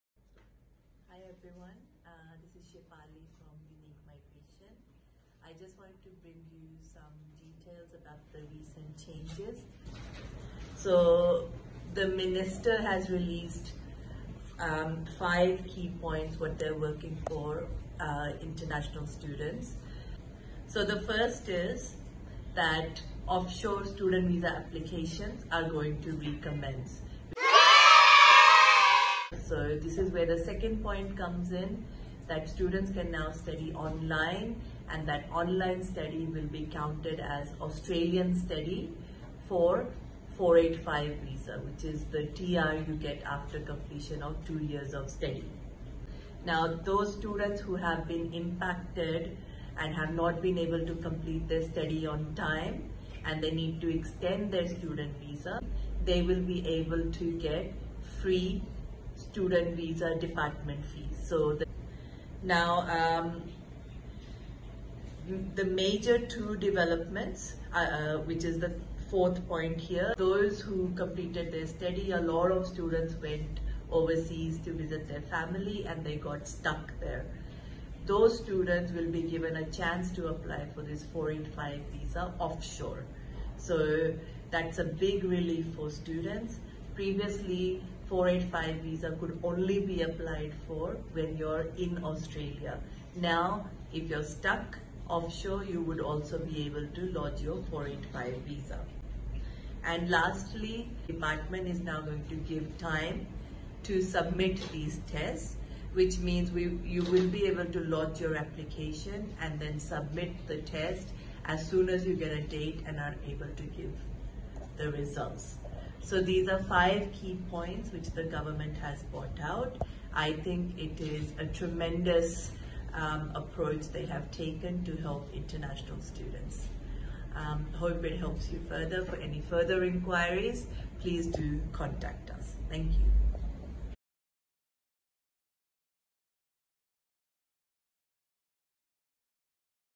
New Australia Student Visa Laws | Changes to Student Visa | Study in Australia
Australia has announced 5  new and important changes that the government is making to help international students already in Australia and those who wish to go to Australia in the near future.
The Corona pandemic has brought the world to a stand still. International students are confused and scared of the uncertainty that lies ahead. Many countries are changing their visa regulations to accommodate international students. Australia's newest regulation amendments show that Australia prioritizes its international students and is willing to do whatever it takes to show that they support their students!

We help students and professionals fulfill their dream of studying and migrating abroad.